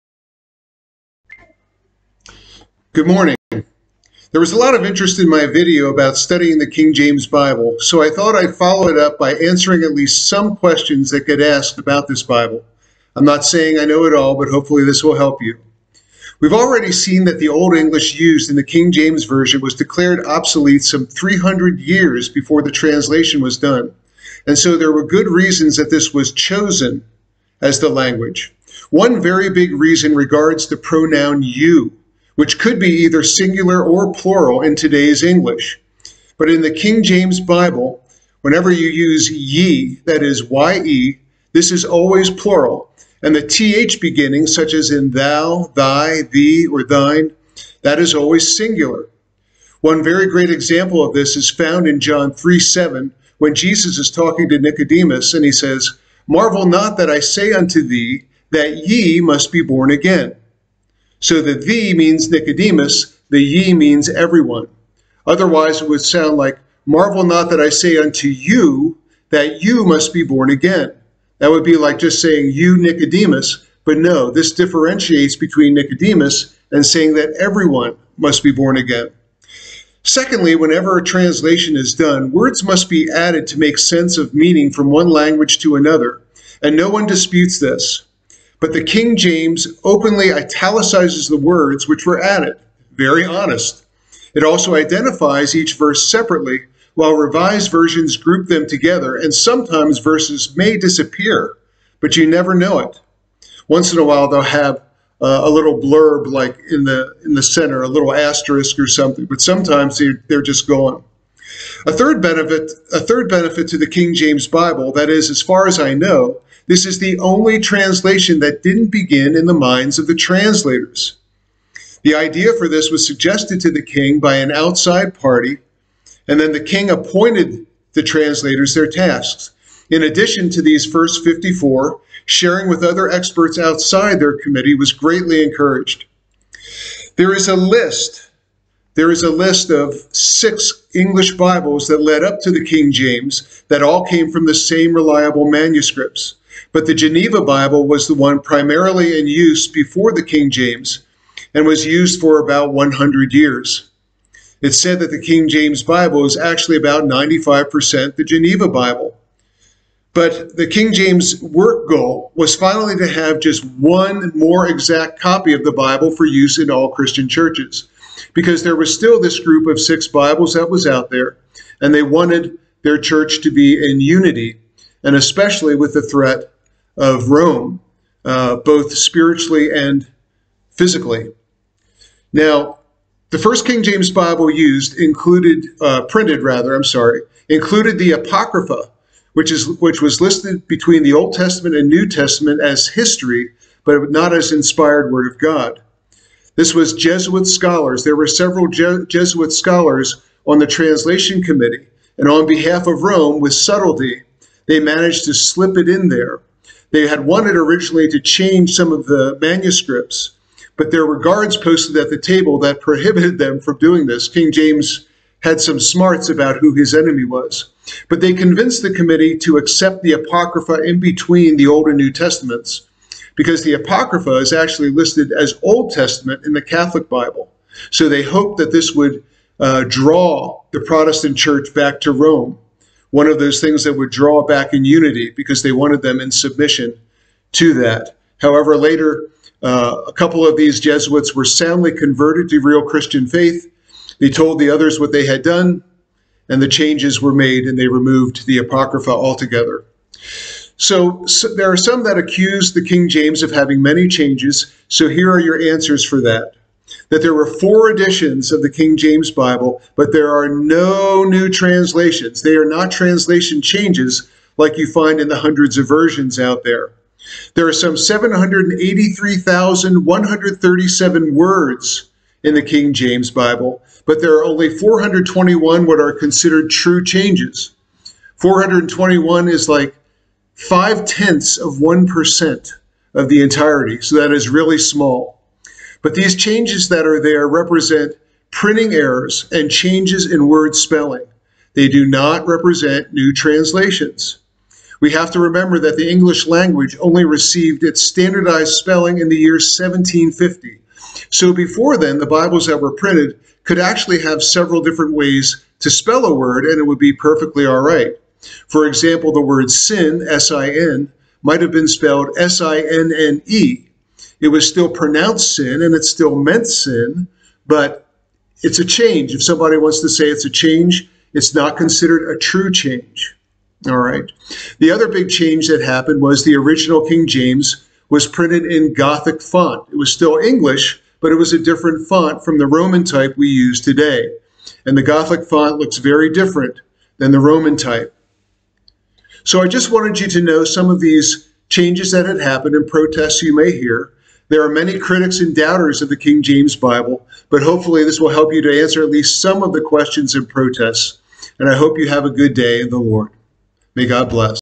Answering Some KJV Questions
These are the scriptures used for this video:
Luke 4:4
John 3:7
2 Timothy 4:3,4
Revelation 22:18,19